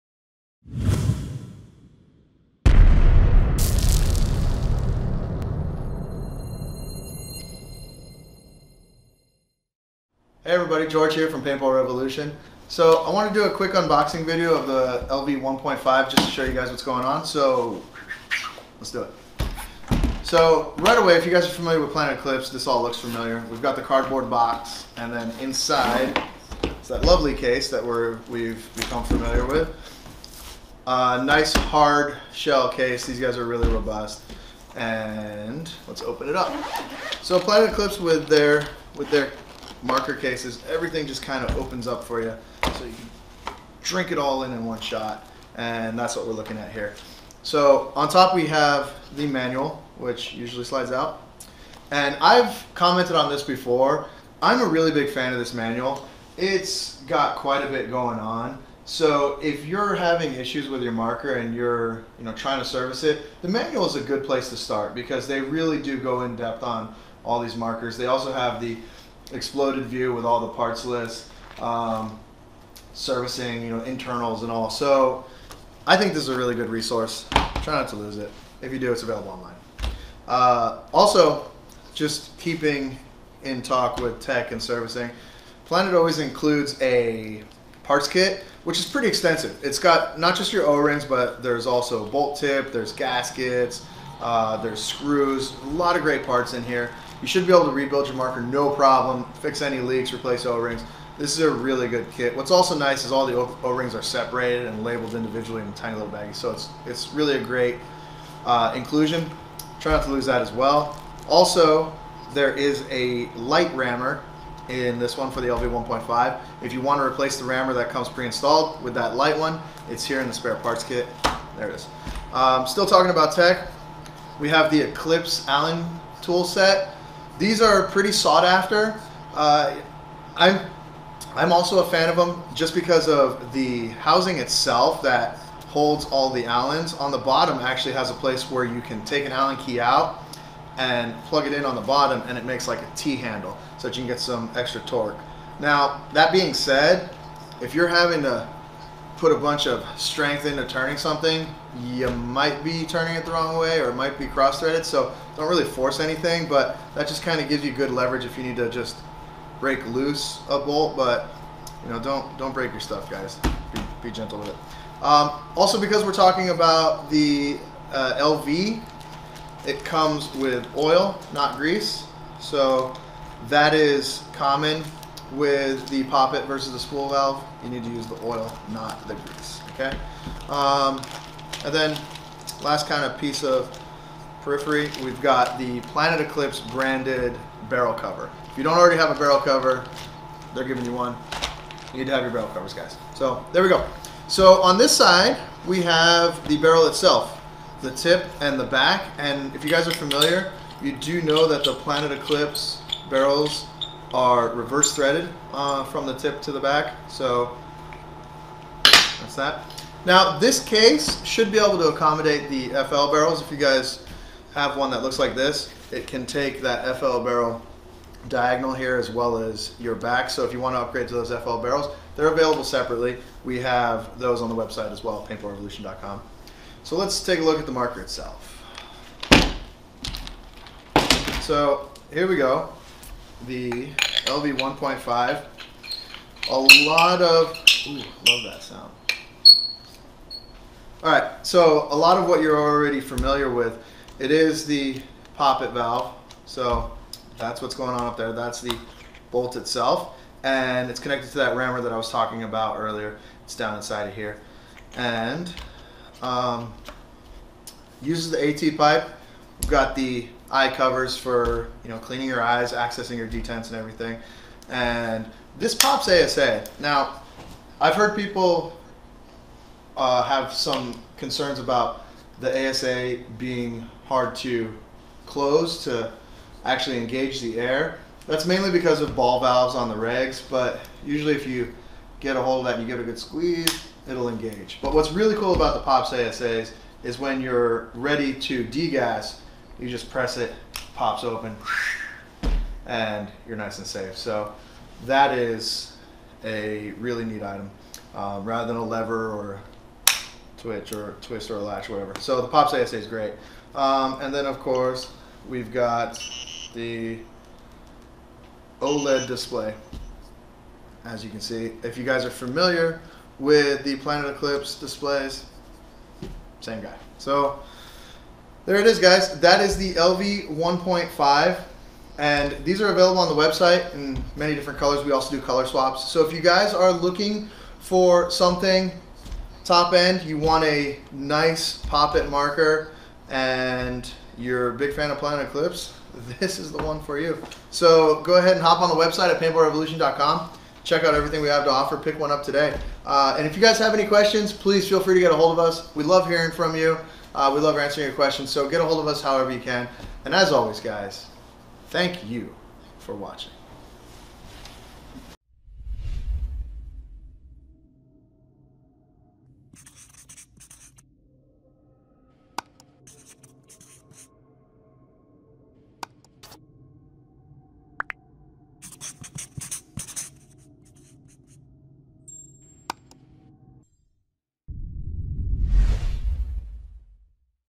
Planet Eclipse Ego LV1.5 Unboxing
In this video, George is unboxing the Planet Eclipse Ego LV1.5 !